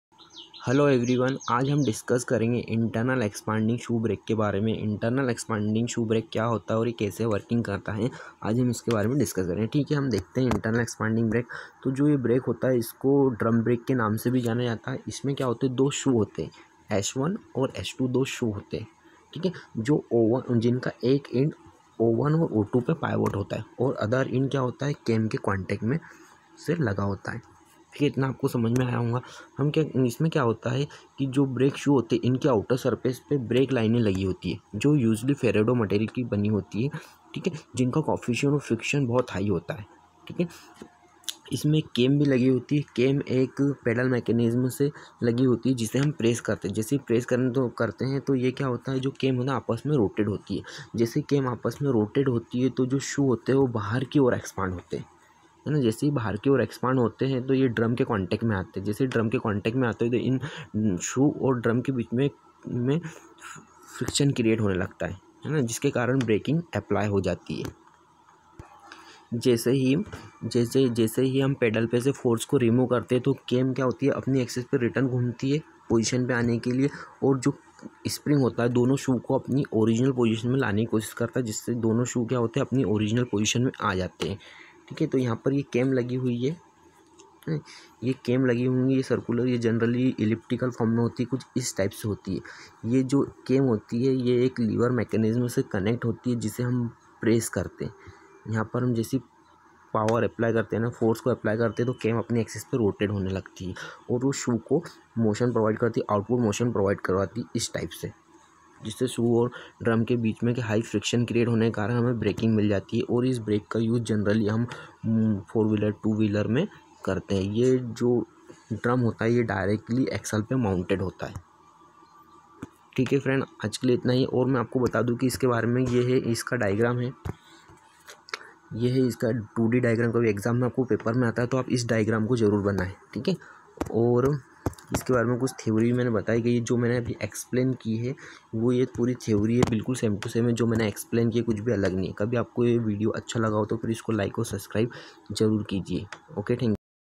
Internal expanding shoe brake
Basic construction and working of internal expanding shoe brake